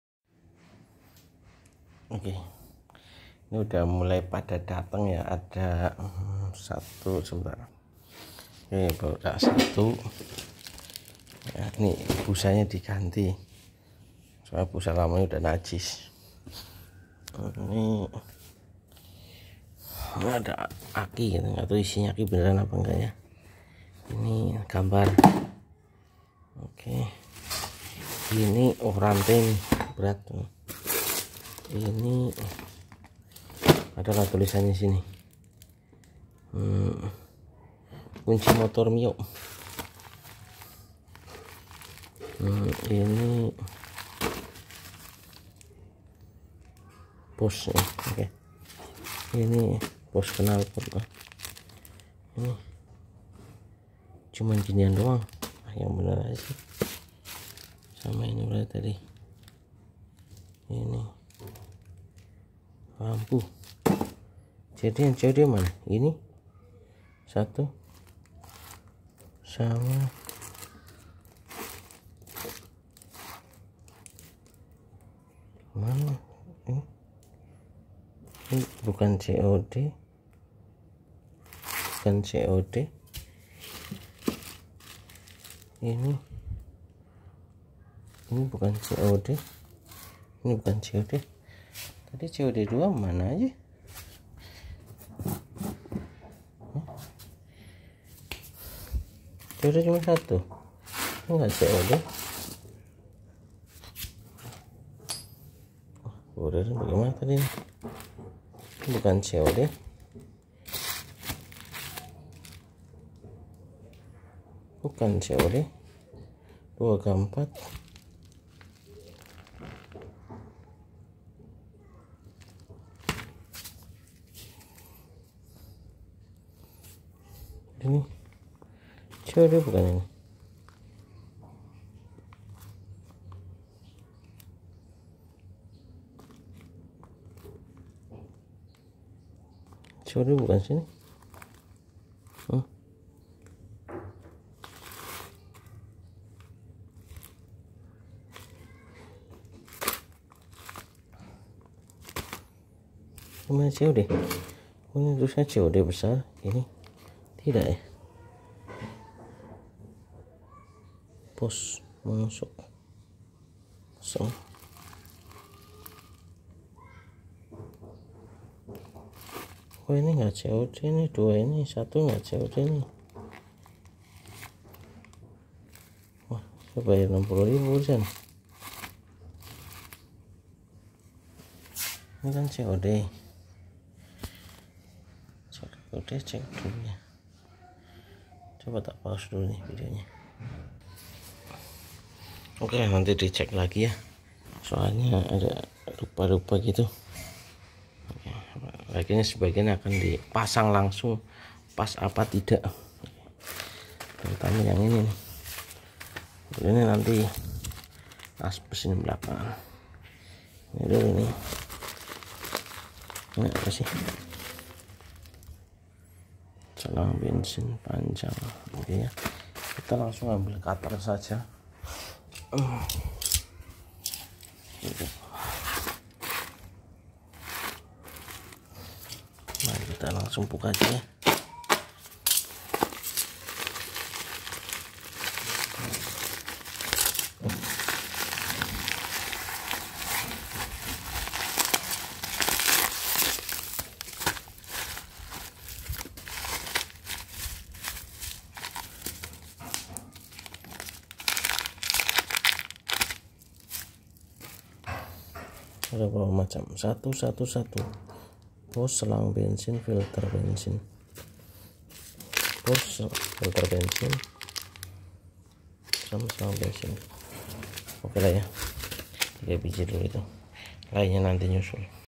Pergantian Sparepart Ninja R 2003 - paket Sparepart ninja R mulai berdatangan
Pembelian ini kami Dokumentasikan agar ketahuan jumlah belanja untuk pergantian dan pemasangan yang tidak ada
Ninja R tahun 2003 lama terbengkelai waktunya di bersihkan dan sebagian diperbaharui